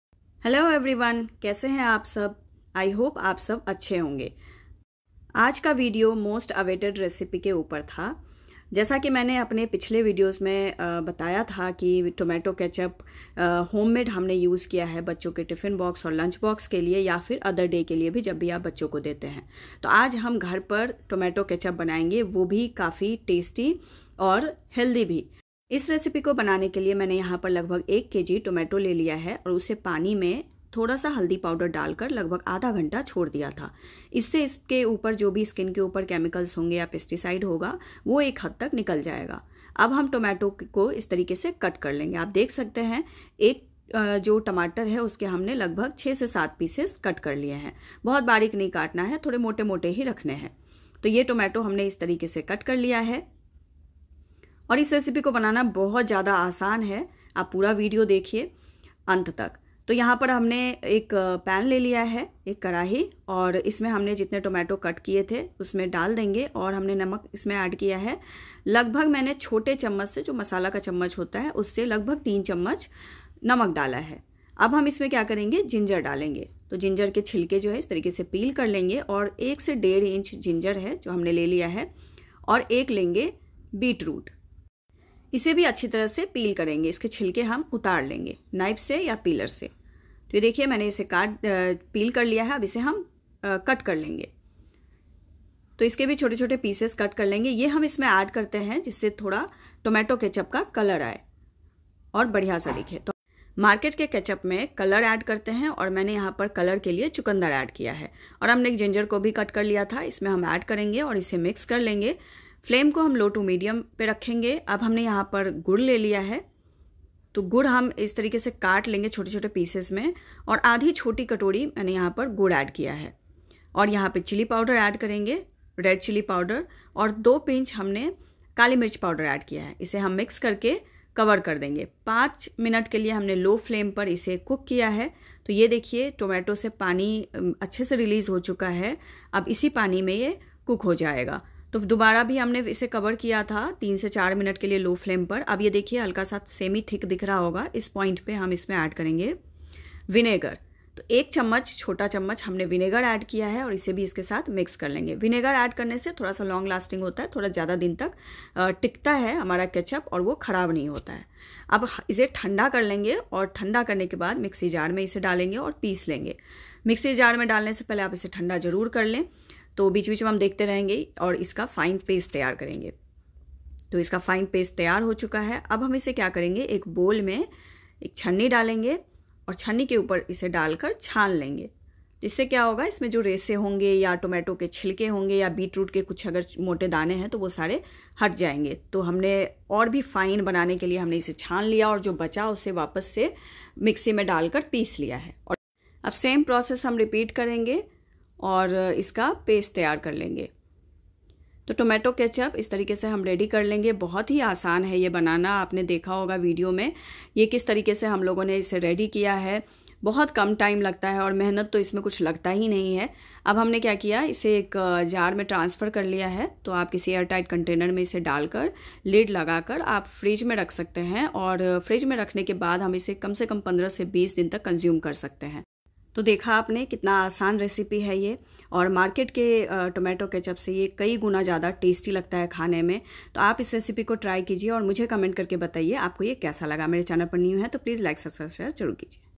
Tomato ketchup quick recipe बिना झिझक बच्चे को healthy Tomato sauce खिलाऐं | Homemade
Ingredients
Red Tomato -1 kg
Beatroot- 1 medium
Ginger- 1 inch
Jaggery- 1/2 bowl
Salt- 2 to 3 small tsp
Red Chilli powder- 1/2 small tsp (optional)

Friends, today I have presented homemade fresh tomato ketchup recipe. It is super easy preparation and super tasty.
Also, it is healthy for everyone. Please watch my video and try this recipe at home
friends.

Say goodbye to store-bought ketchup full of additives and hello to a healthier, tastier alternative. Get ready to elevate your meals with the perfect balance of sweet and savory, all from scratch. Don't miss out on this satisfying recipe that will have everyone asking for more. Hit play now and get saucy!"